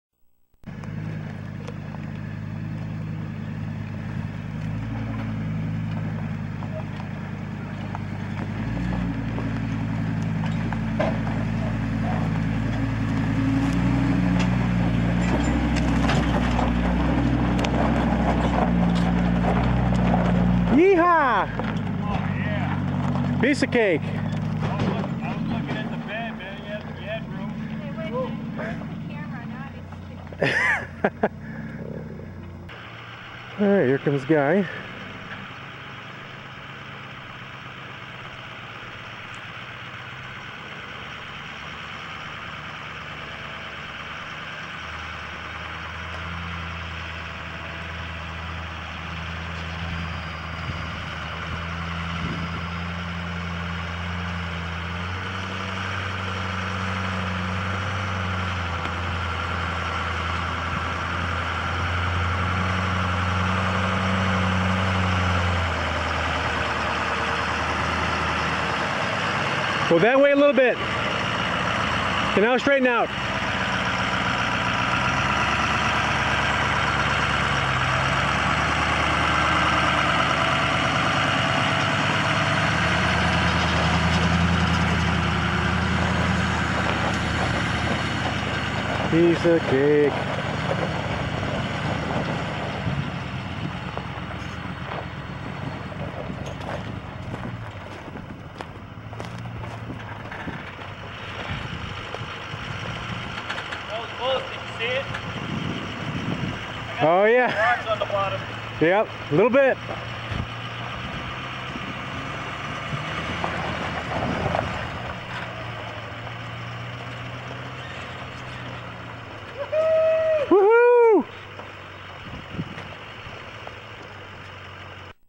Mountain Camping
Pulling RV trailers through steep mountain trail